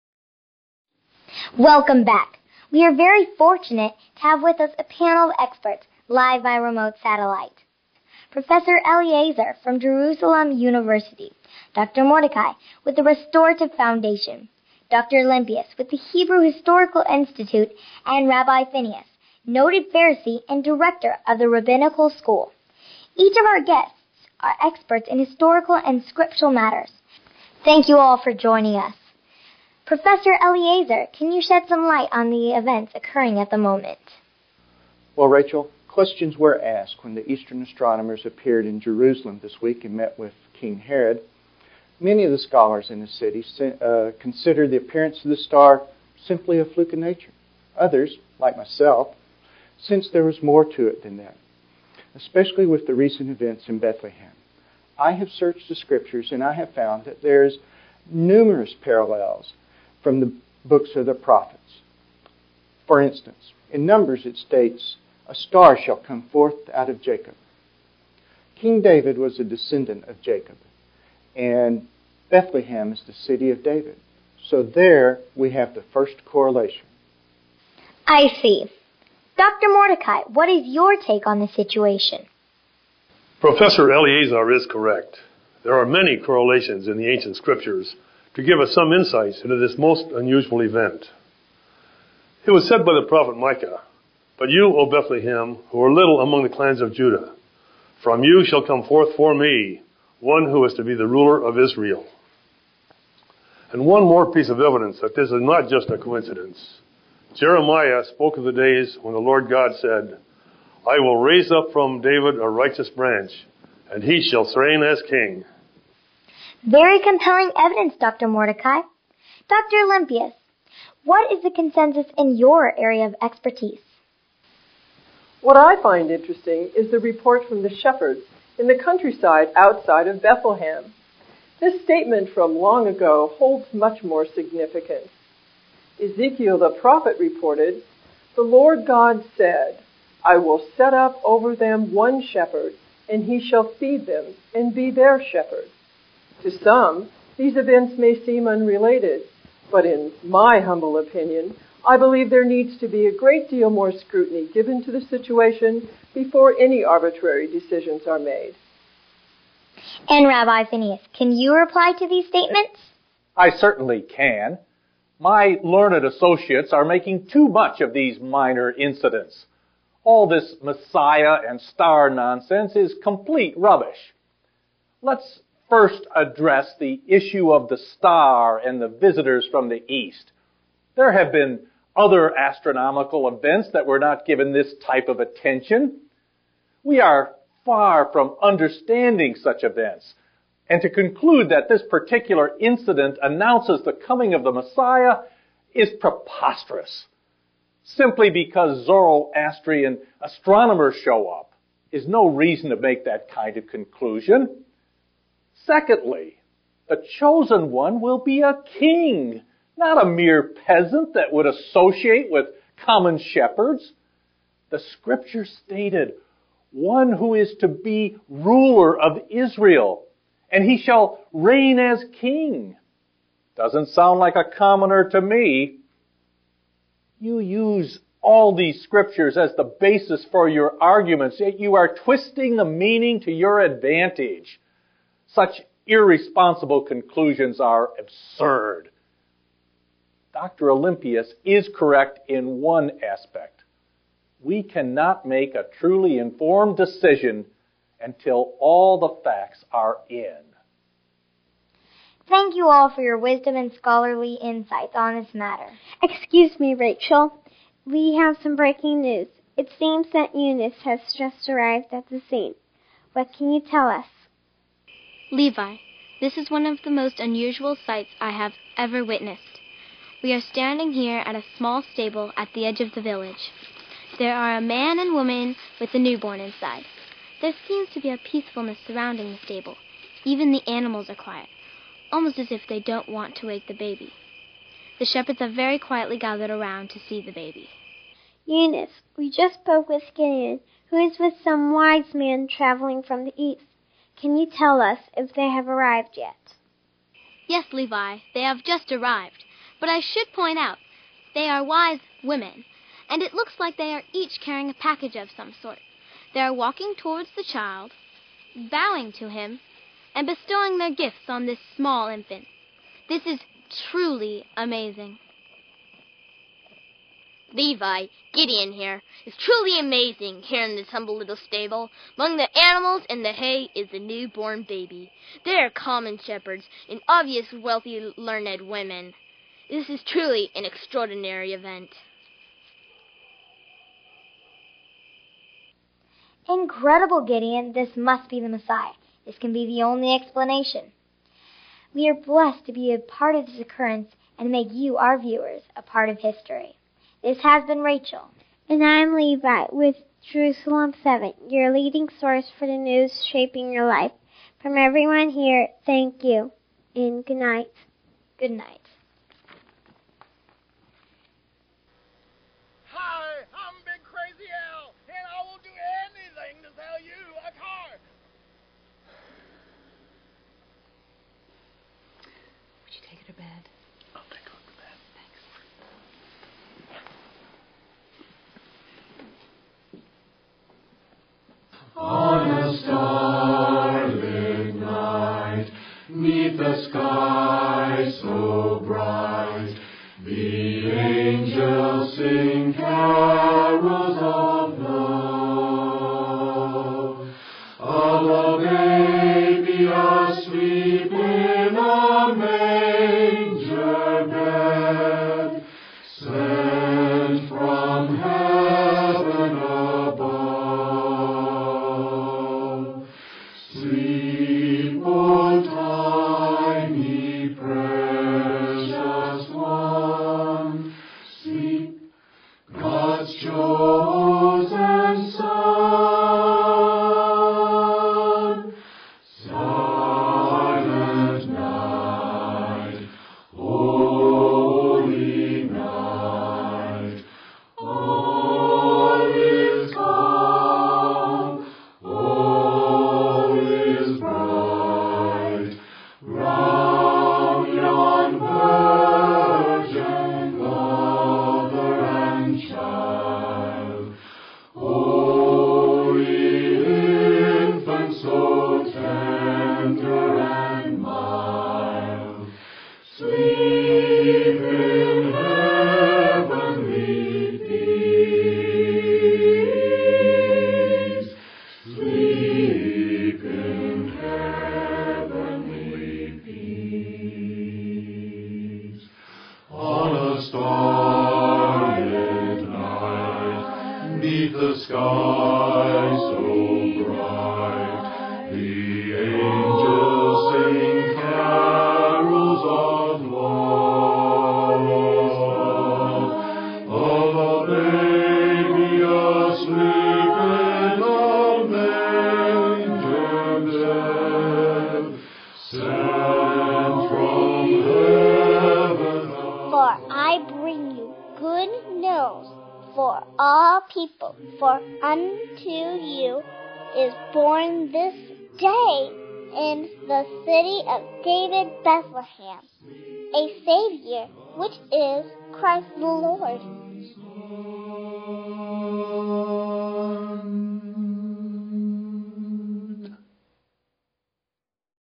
Good News Broadcast Part 3
A child falls asleep on Christmas Eve to the 10 o'clock news and awakens to a news broadcast of the Christmas Story with child newscasters.